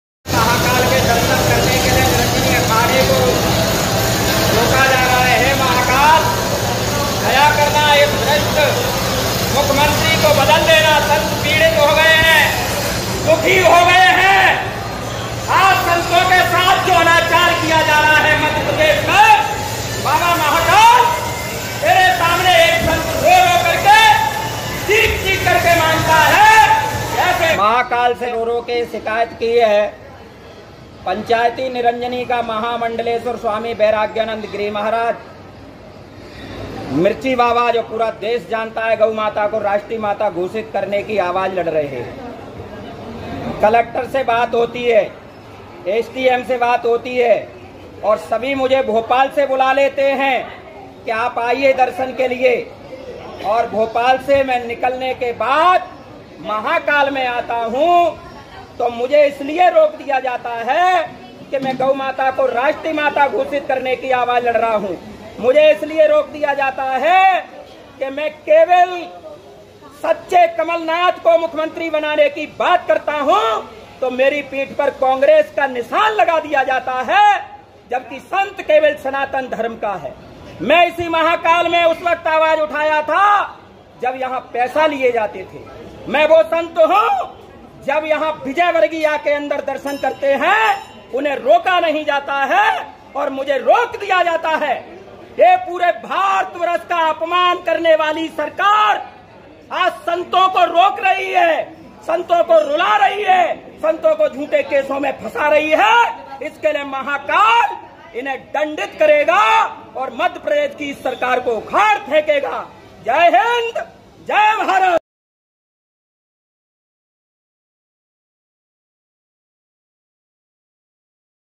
महाकाल मंदिर में मिर्चीबाबा का हंगामा,गर्भगृह में नही मिला प्रवेश तो चिल्लाने लगे बाबा
उज्जैन महाकाल मंदिर में मिर्ची बाबा का हंगामा,गर्भगृह में नही मिला प्रवेश तो चिल्लाने लगे बाबा,कहा महाकाल सीएम को बदल दो